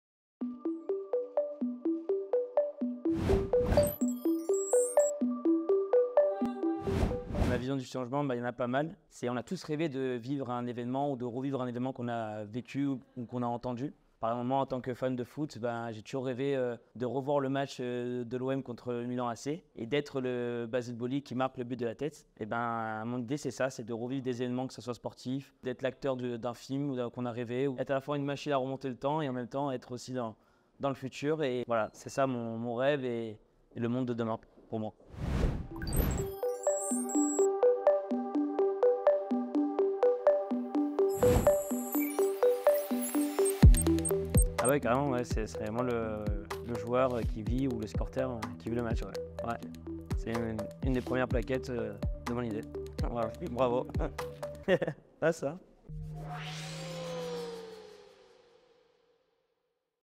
Prompt The Change - Léo
Le sens porté par la nouvelle signature de KEDGE, “Let’s be the change” incarne la vocation de l’école d’être une entreprise du changement. Le changement est une notion qui peut être difficile à se représenter, et qui peut être vu sous une multitude d’angles. Chaque individu à sa propre conscience et sa propre conception du changement. C'est pourquoi KEDGE a demandé à ses étudiants d'exprimer leur propre vision du changement. Une vision qui prendra vie grâce à une image générée par l'intelligence artificielle et sur laquelle ils seront invités à réagir. Une manière simple d'utiliser l'IA au service de la représentation de notre futur imaginé par nos étudiants. KEDGE, let's be the change